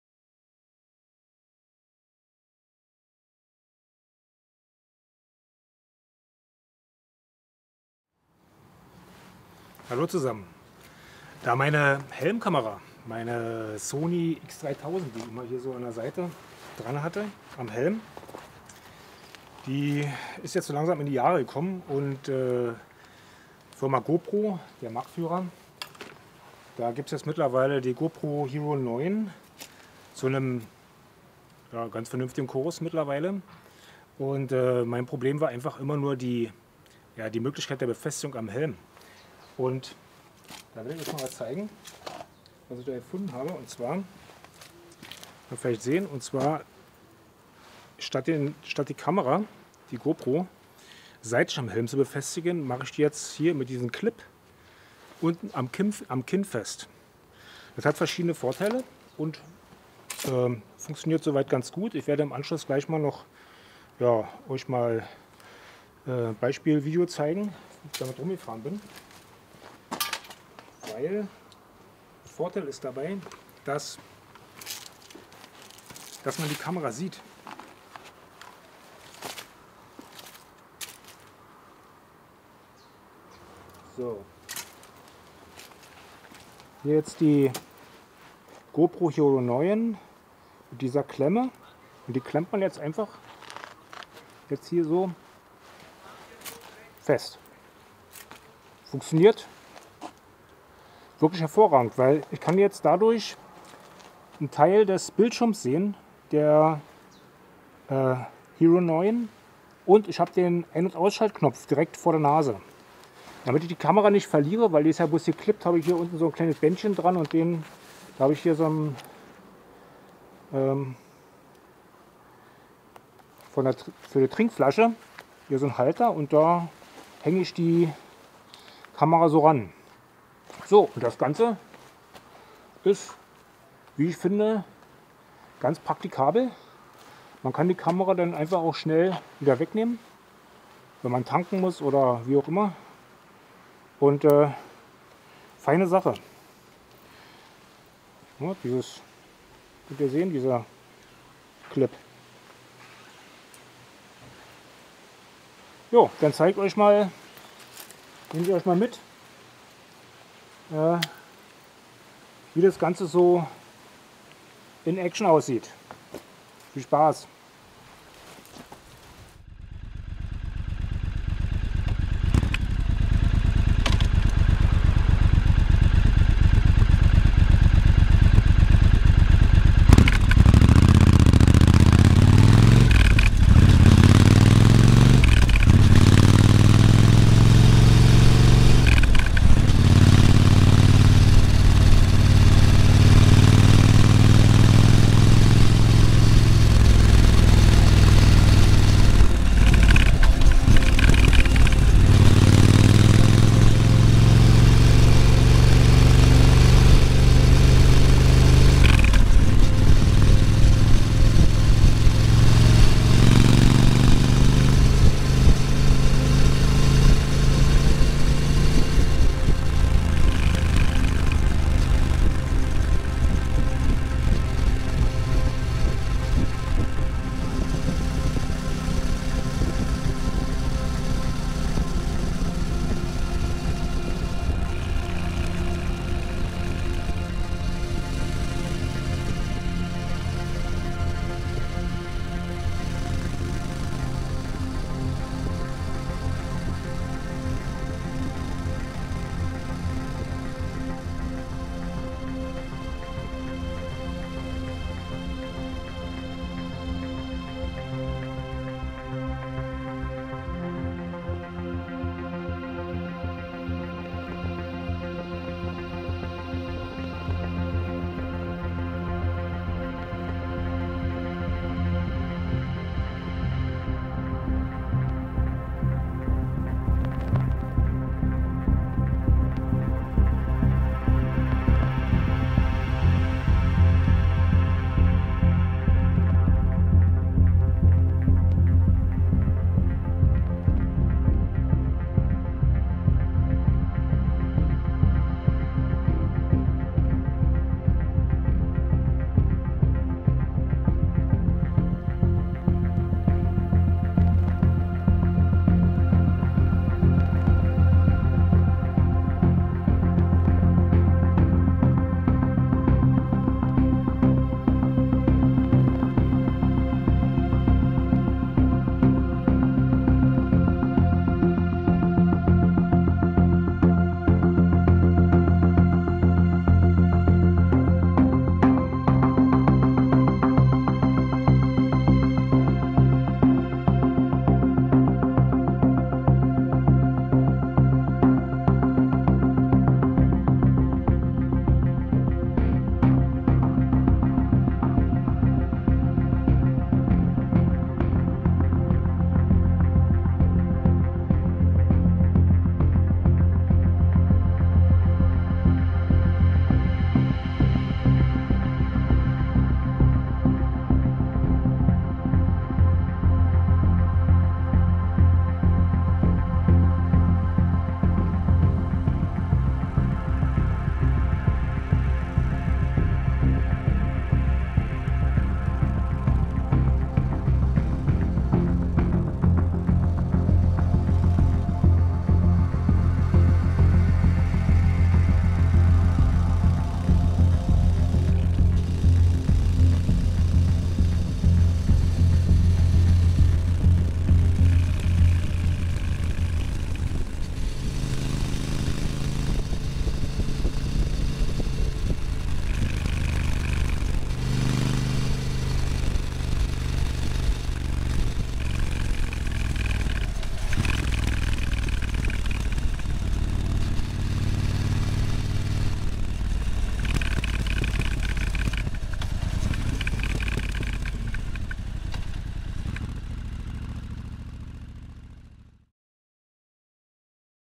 GoPro Hero Motorrad Helmbefestigung | Dango Design Gripper | 🎧 DIY Musik
Hallo und Servus. Wer keine Löcher bohren will XD oder kleben mag, hat ein Problem, eine Action Kamera am Helm sinnvoll zu befestigen. Hier ein kleiner Tipp, wie es gehen könnte.
Grüße aus Franken
rumo ✌️